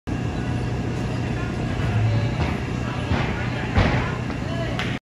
FHS-FL-FL 10/13/2020
Working front tumbling, getting some power finally! ☺️ October 13, 2020, age 13, level 10. SCEGA.